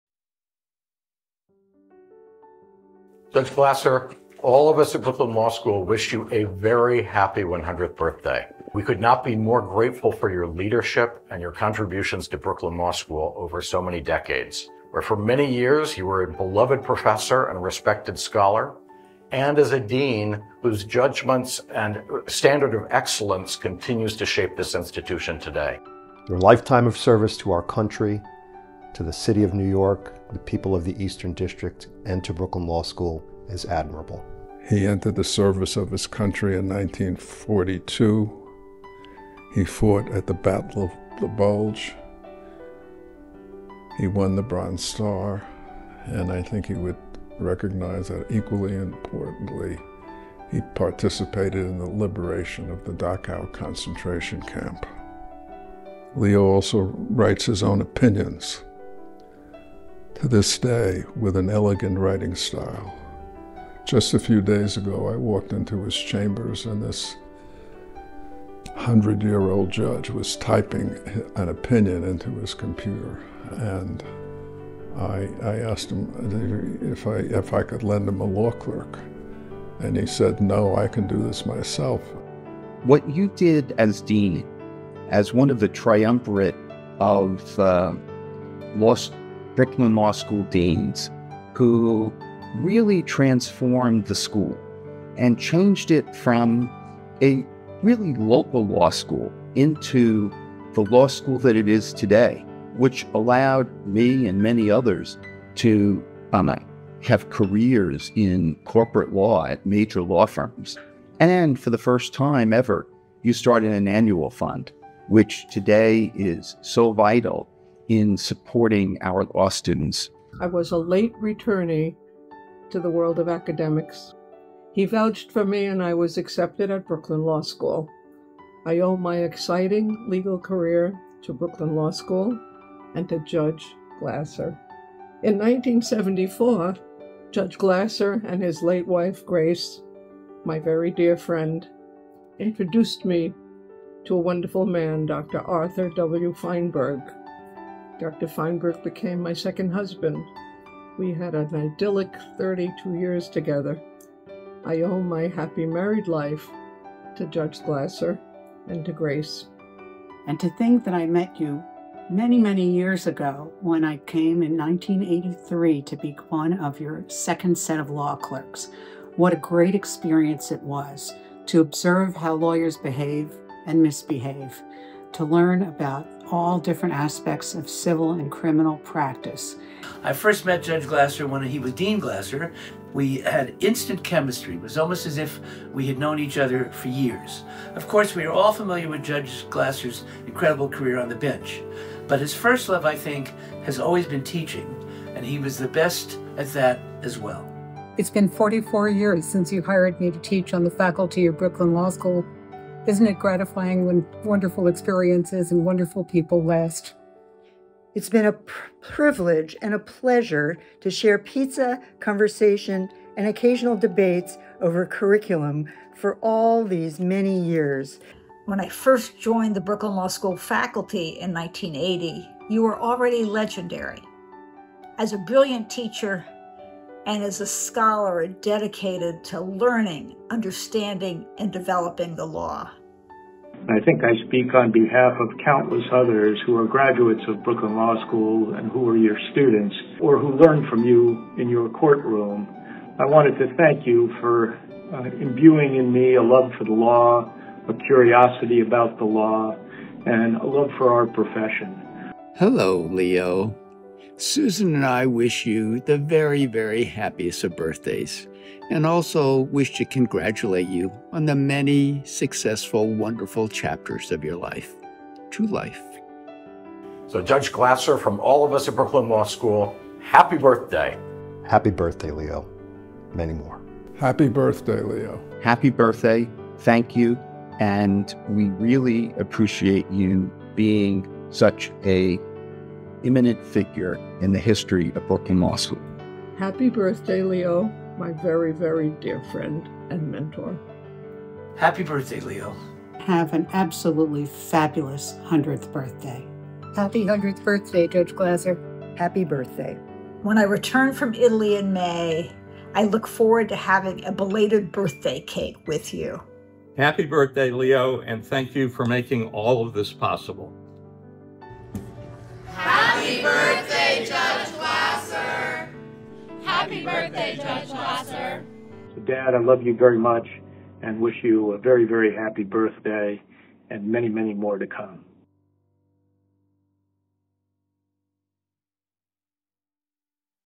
Happy 100th Birthday, Judge Leo Glasser!
The Brooklyn Law community wishes a happy 100th birthday to the Hon. I. Leo Glasser ’48, U. S. District Court Judge of the Eastern District of New York, brilliant alumnus and scholar, beloved professor and mentor, and our distinguished former dean.